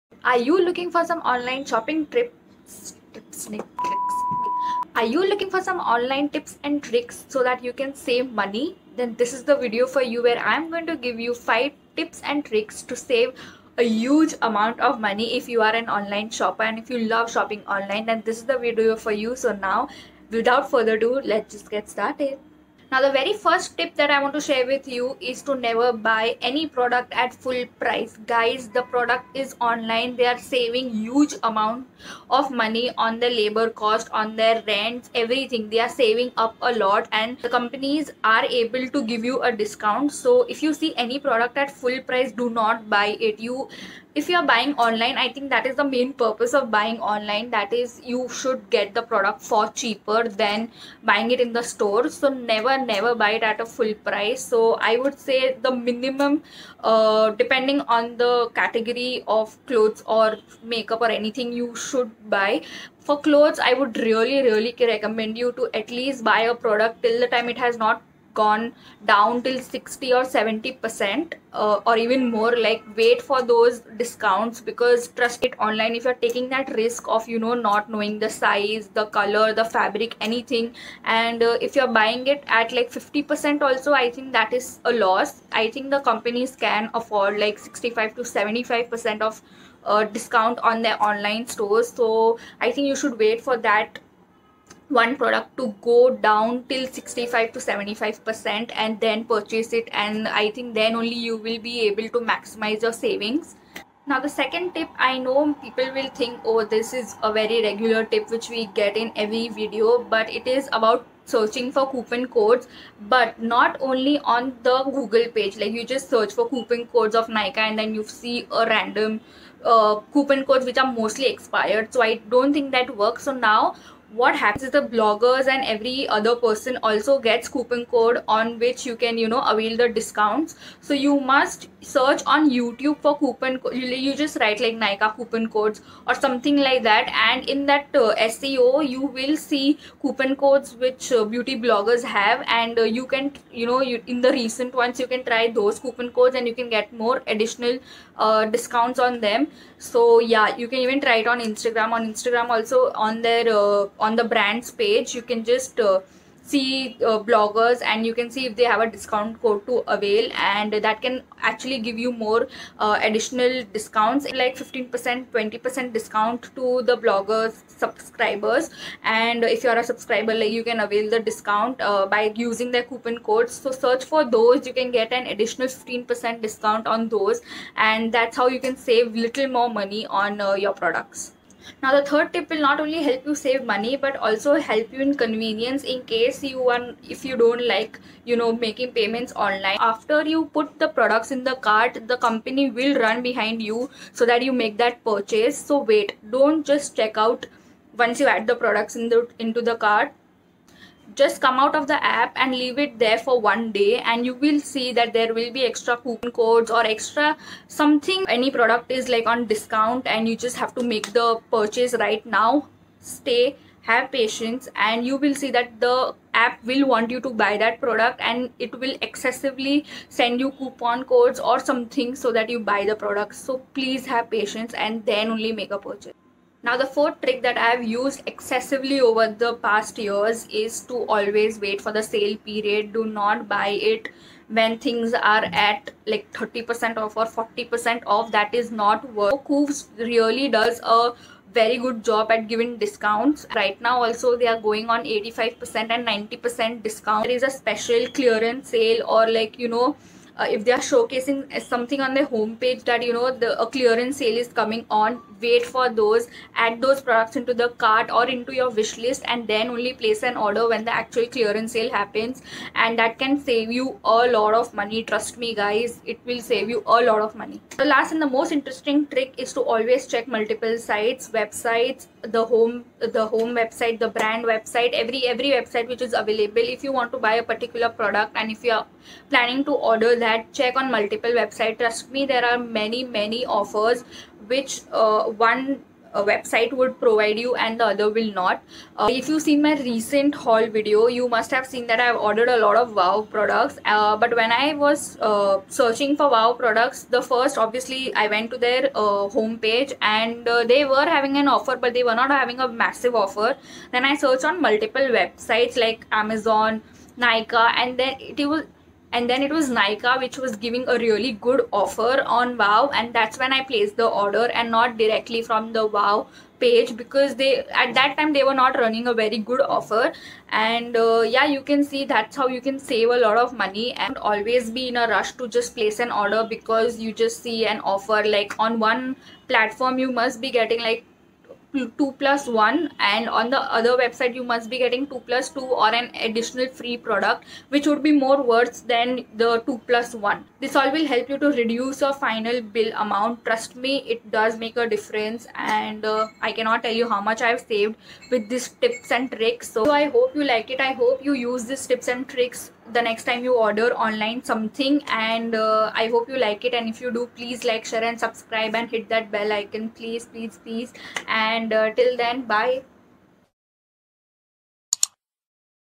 TOP UNIQUE TIPS TO SAVE MONEY WHILE ONLINE SHOPPING |SAVE BIG ON MYNTRA ,KOOVS,AJIO ,NYKAA ETC
DO YOU WANT TO SAVE A LOT OF MONEY WHILE SHOPPING ONLINE FROM MYNTRA,KOIVS,NYKAA ,AJIO ETC ?!?
Watch this vidoe to learn some unique tips & tricks to save extra money while Shopping online ,trust me this are not your usual ones !!

Watch my recent haul vidoes where I shop at some Crazy discounts online :